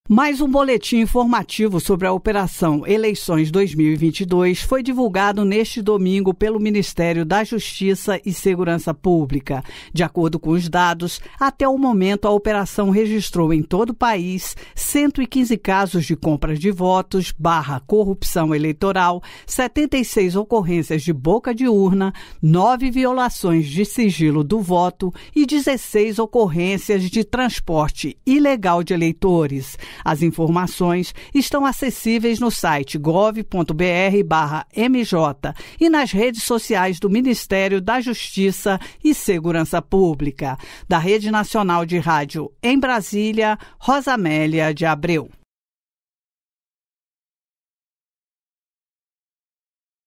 2/10/2022 - NOVO BOLETIM SOBRE AS ELEIÇÕES REGISTRA 115 CASOS DE COMPRA DE VOTOS
Dados do ministério da Justiça e Segurança Pública informam ainda 76 ocorrências de boca de urna, 9 violações de sigilo do voto e 16 ocorrências de transporte ilegal de eleitores.

Mais um boletim informativo sobre a Operação Eleições 2022 foi divulgado neste domingo pelo ministério da Justiça e Segurança Pública .

De acordo com os dados , até o momento, a Operação registrou em todo o país:

115 casos de compra de votos/ corrupção eleitoral;

• 76 ocorrências de boca de urna;

• 9 violações de sigilo do voto; e

• 16 ocorrências de transporte ilegal de eleitores.

registrado em: boletim Operação Eleições 2022 ministério da Justiça e Segurança Pública